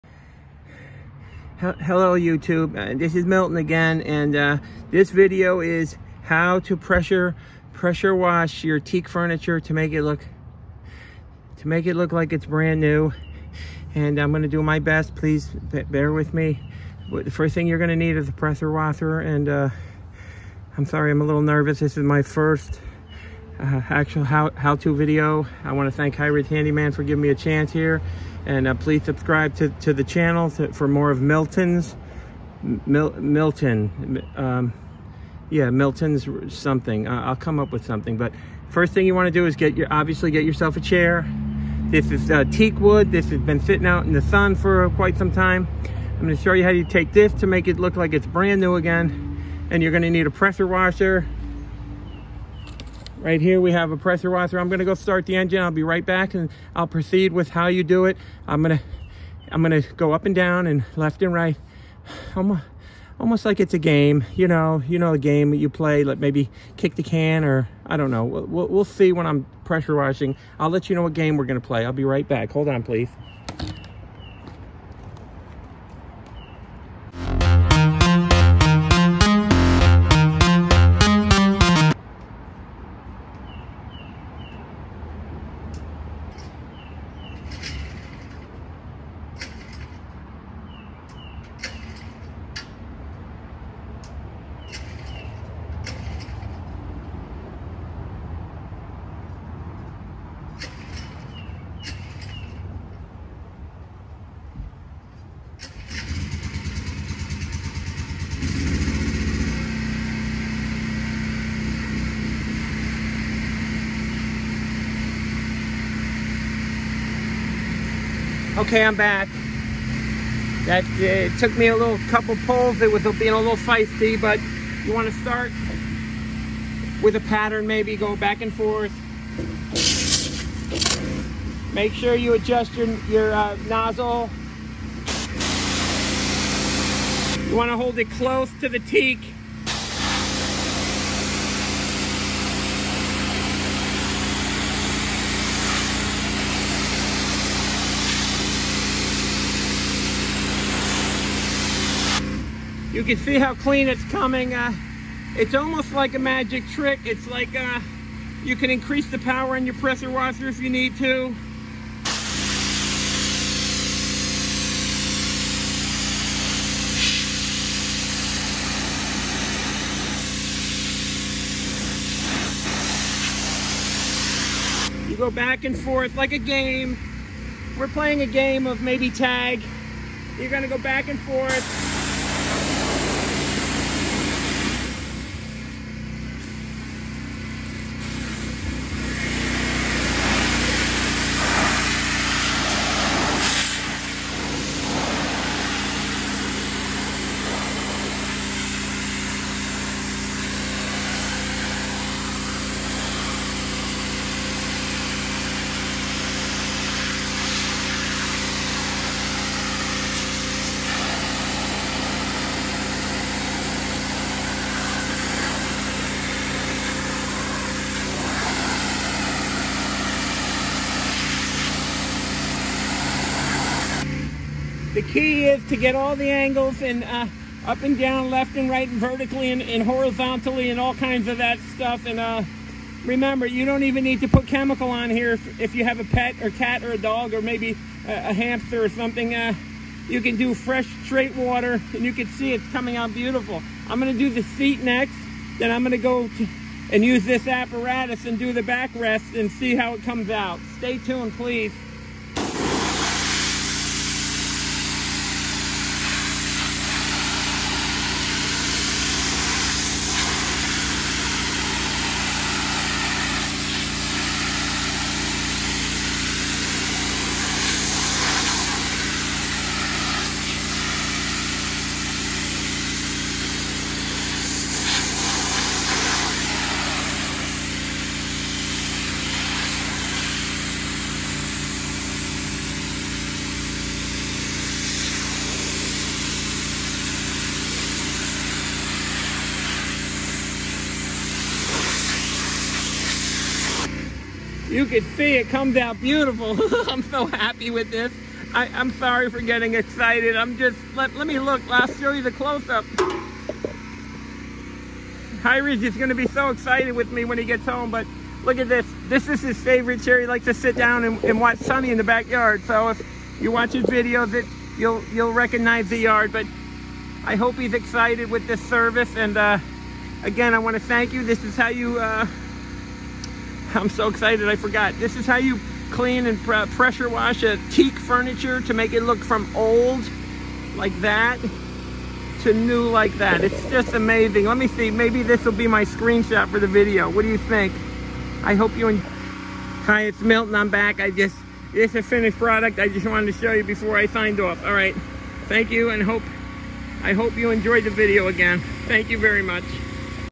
Milton’s How to restore your Teak Furniture with a simple wash
Milton shows you how to restore your old teak wood furniture back to new condition using only hose water